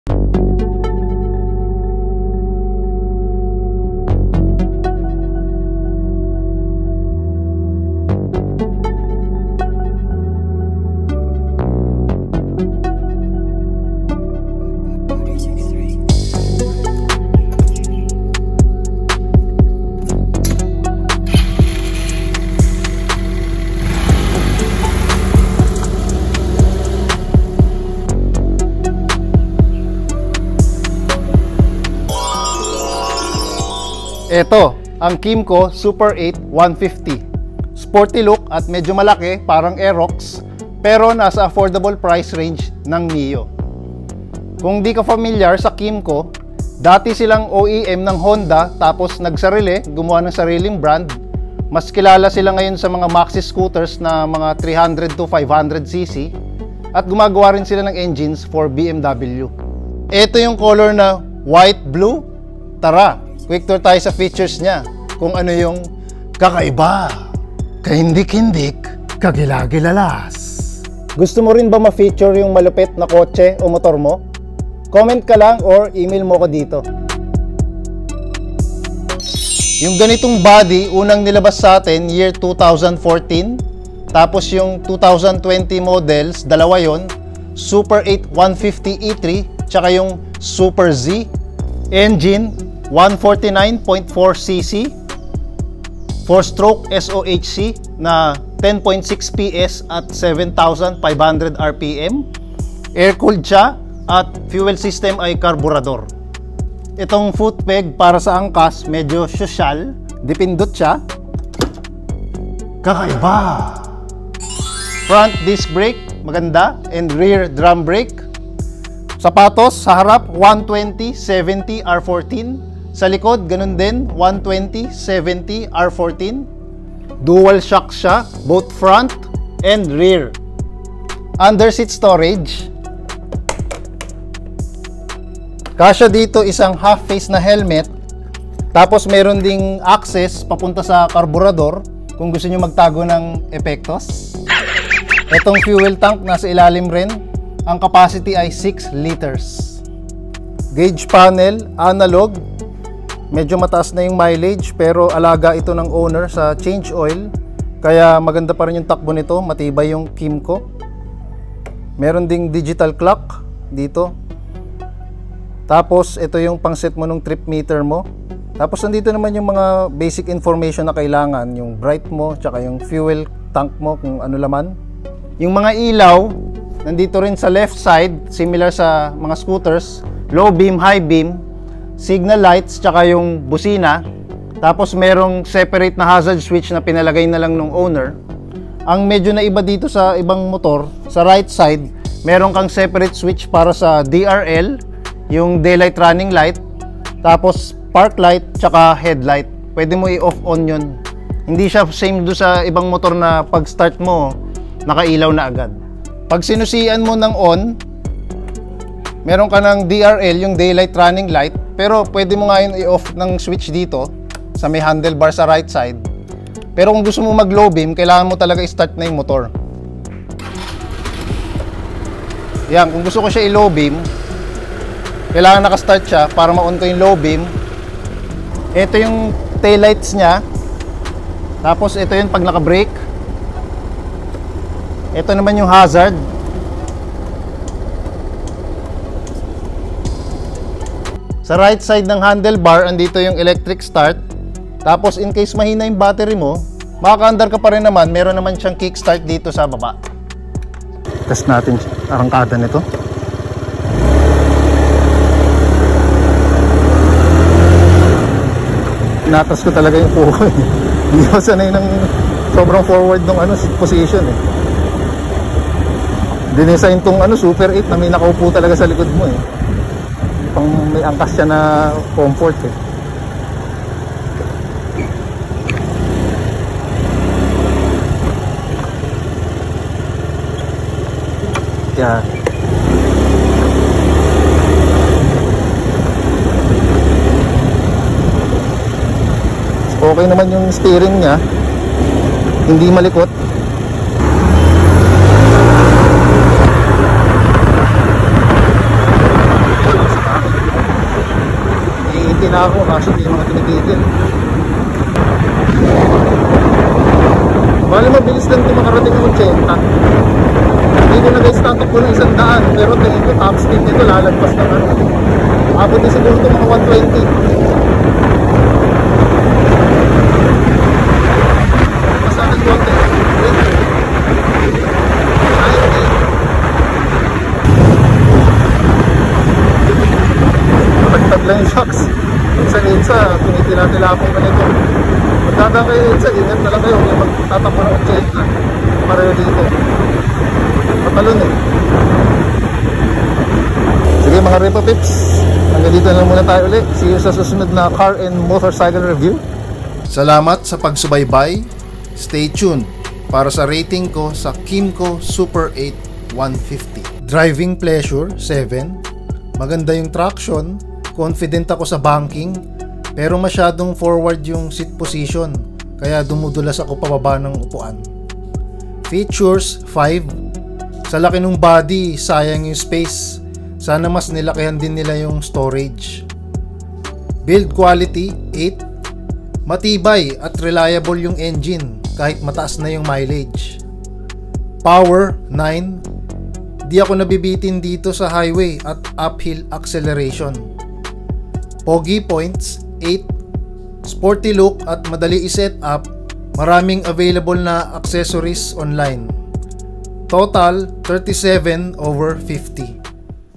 Kymco Super 8 150 review at road test ng sporty scooter
Kymco Super 8 150cc review at road test

+ specs and engine review
+ features, seat storage, headlights, gauge panel
+ road test at highway acceleration